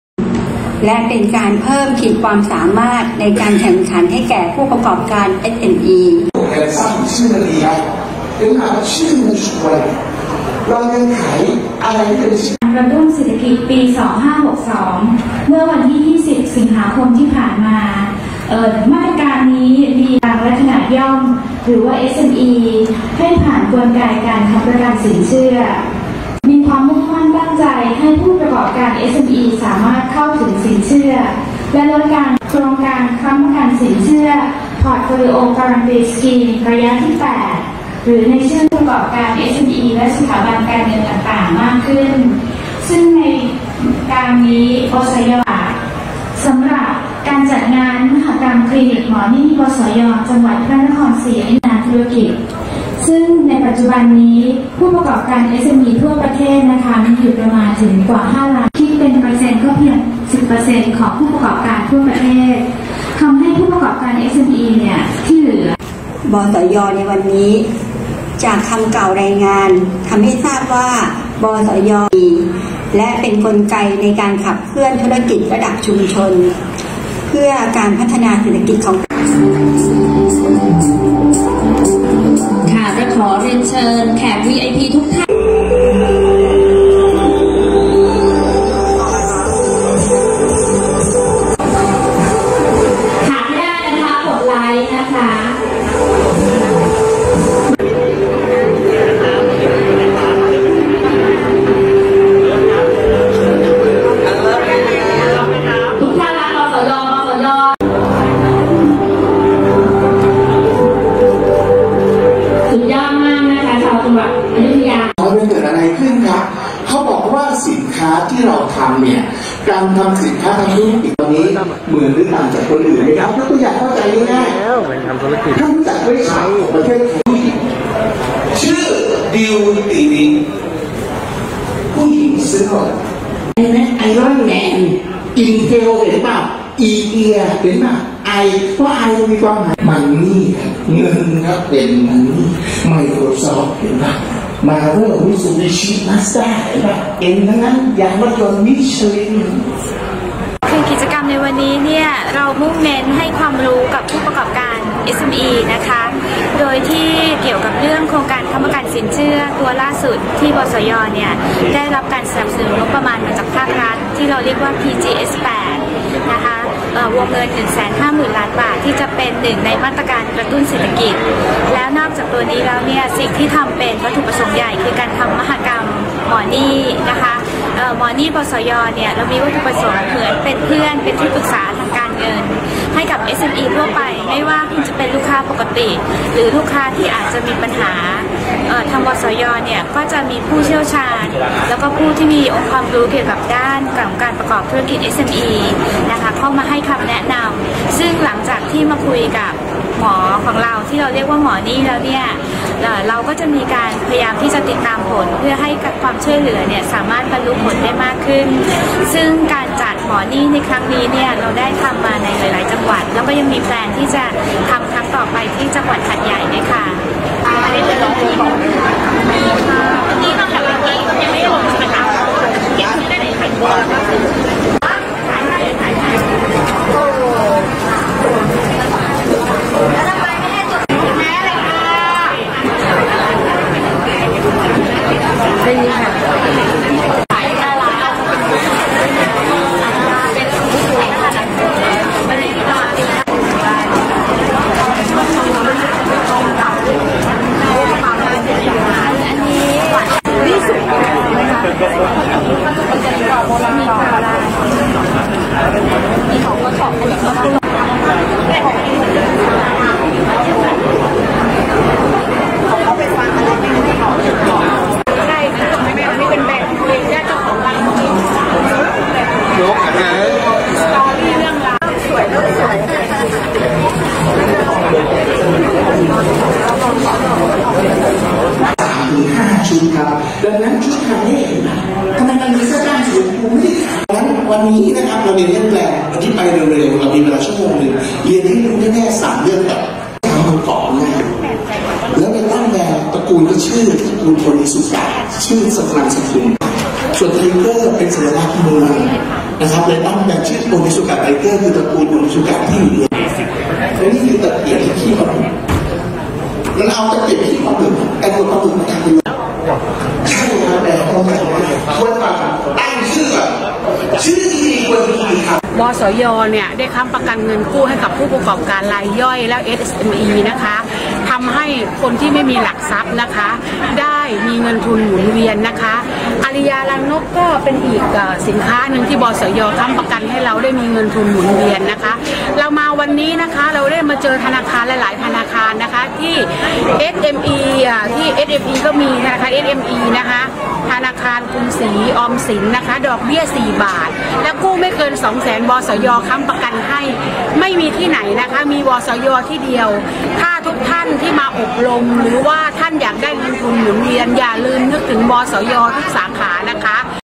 บสย.เปิดมหกรรมคลินิกหมอหนี้ บสย.ภาคกลาง เดินหน้าช่วยเหลือผู้ประกอบการธุรกิจ SMEs
15/10/2019 บสย.เปิดมหกรรมคลินิกหมอหนี้ บสย.ภาคกลาง เดินหน้าช่วยเหลือผู้ประกอบการธุรกิจ SMEs

พระนครศรีอยุธยา - บสย.เปิดมหกรรมคลินิกหมอหนี้ บสย. สำนักงานเขตภาคกลาง เดินหน้าช่วยเหลือผู้ประกอบการธุรกิจ SMEs ที่กำลังต้องการสินเชื่อเพื่อเสริมสภาพคล่องและขยายธุรกิจ พร้อมกิจกรรมและการบรรยายให้ความรู้แก่ผู้ประกอบการ SMEs จัดเต็มทั้งเติมทุน เติมความรู้ และเติมคุณภาพชีวิต